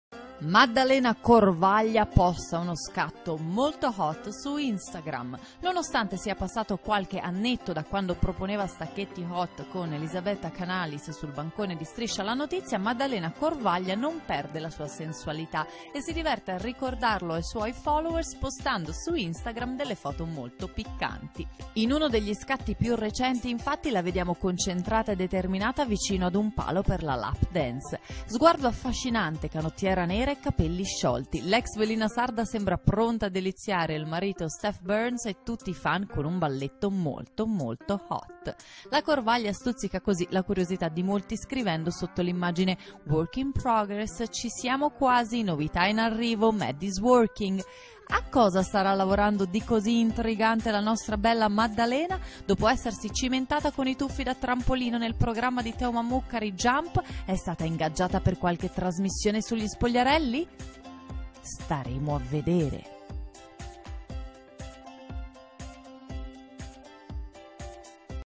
Maddalena Corvaglia, ex velina sexy mentre balla la lap dance
Maddalena Corvaglia posta uno scatto hot su Instagram